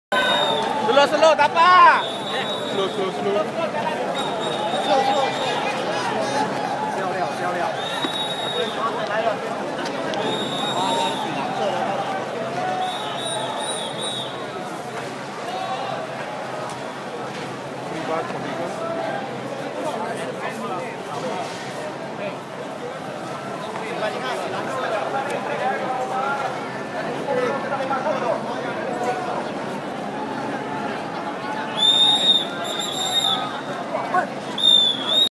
In comes the coloured water canons...
If the tear gas wasn't enough, there were water cannons which had been dyed with blue colour to "cool" down the Bersih supporters